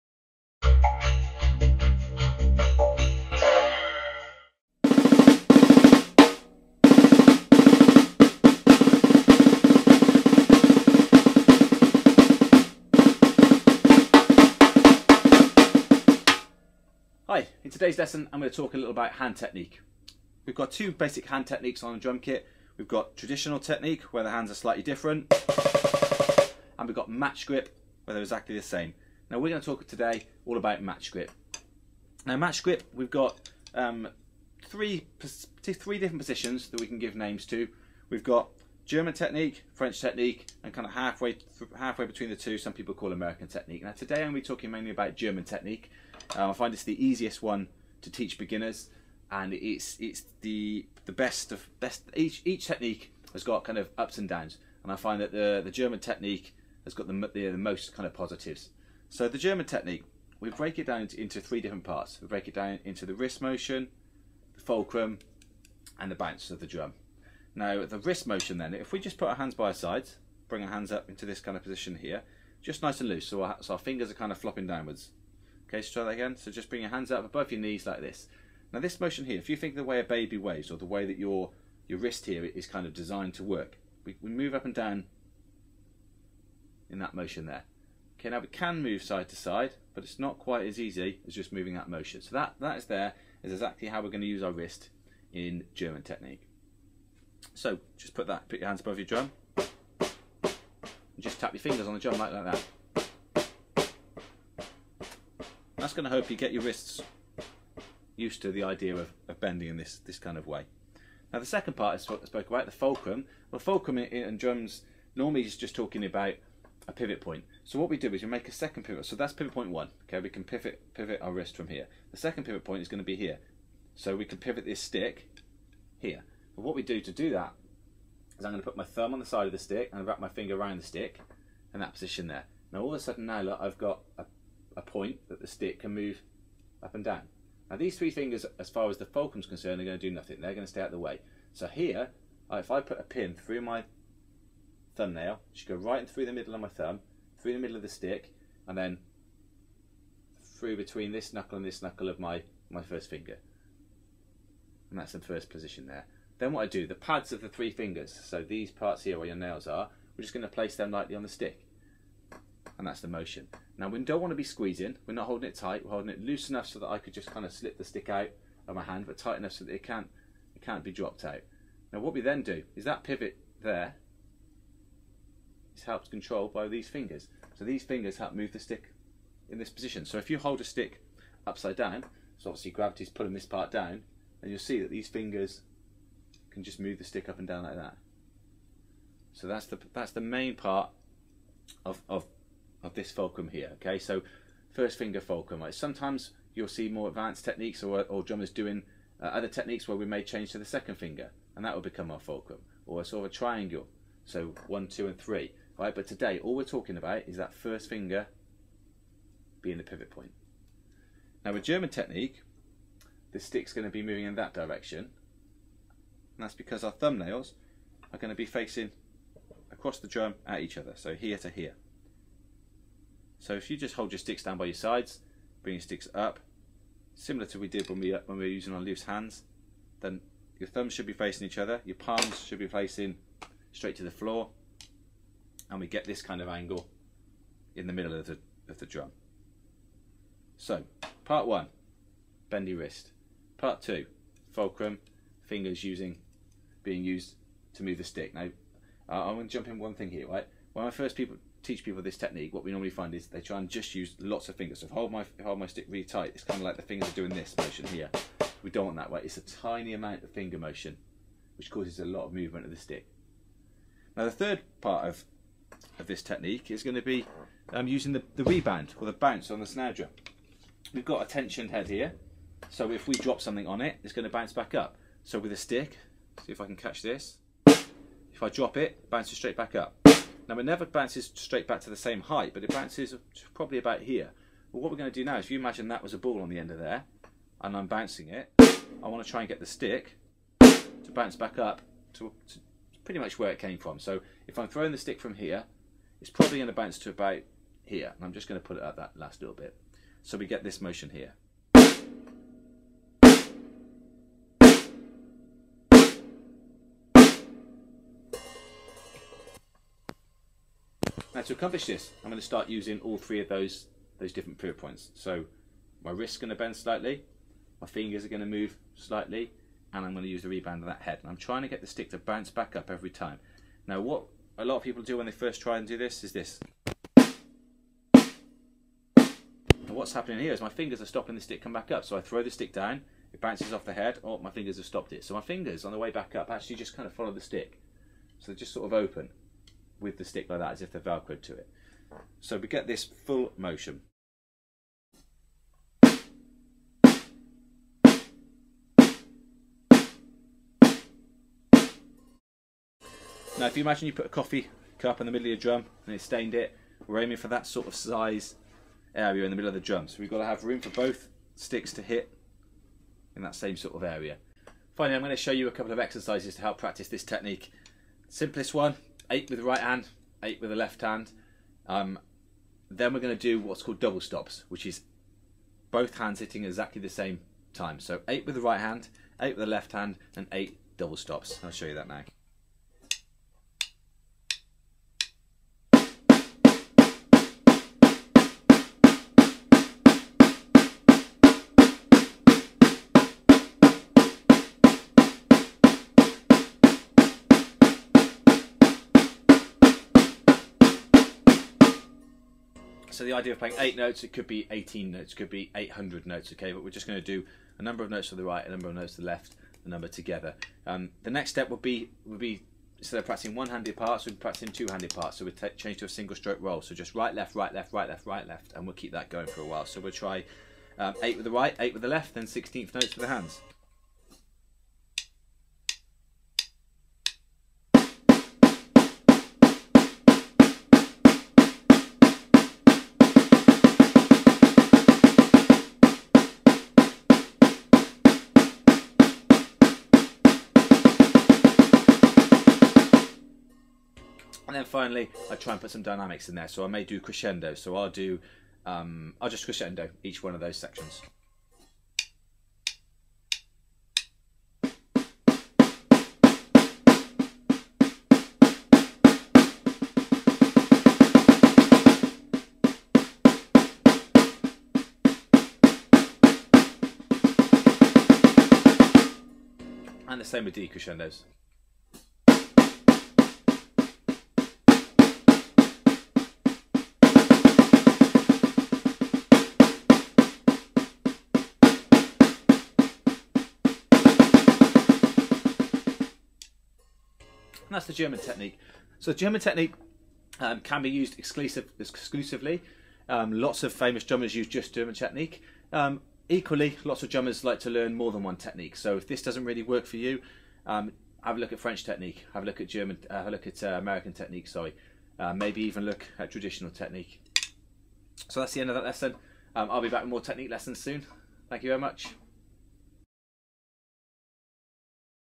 Free drum lesson How to hold Drum Sticks - Basic Drummers hand technique lesson
This is a free drum lesson on how to hold drum sticks in the basic Greman Technique.
For more technique lessons check out idrumtutor.com